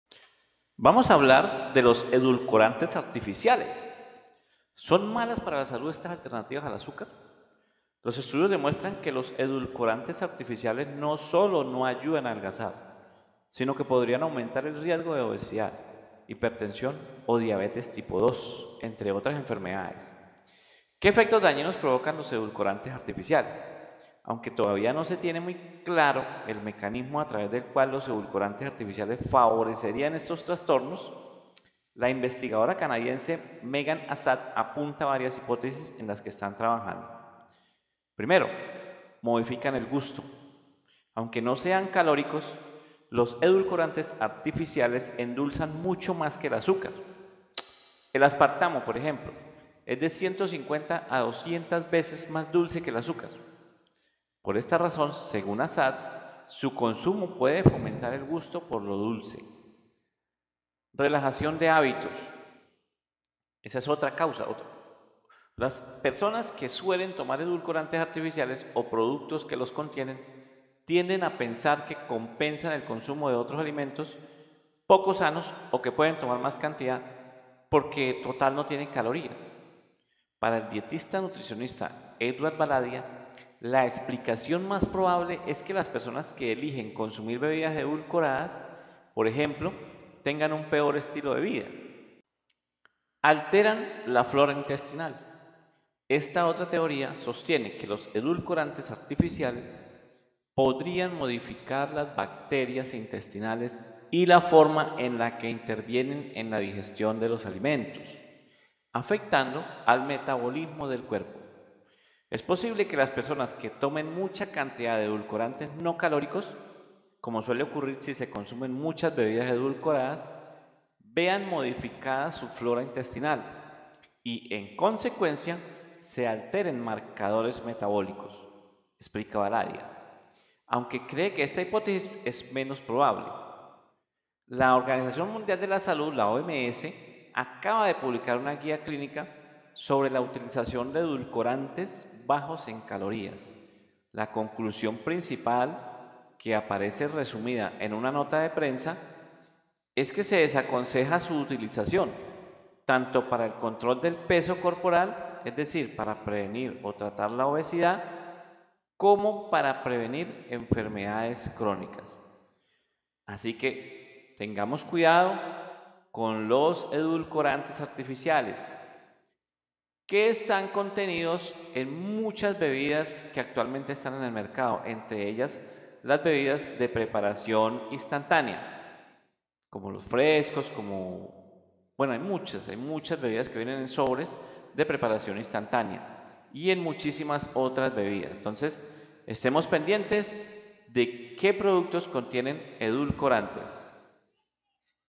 RIESGO DE LOS EDULCORANTES ARTIFICIALES
Los estudios demuestran que los edulcorantes artificiales no solo no ayudan a adelgazar, sino que podrían aumentar el riesgo de obesidad, hipertensión o diabetes tipo 2, entre otras enfermedades. El consumo de edulcorantes no solo no contribuye a la pérdida de peso, sino que a la larga podría favorecer la obesidad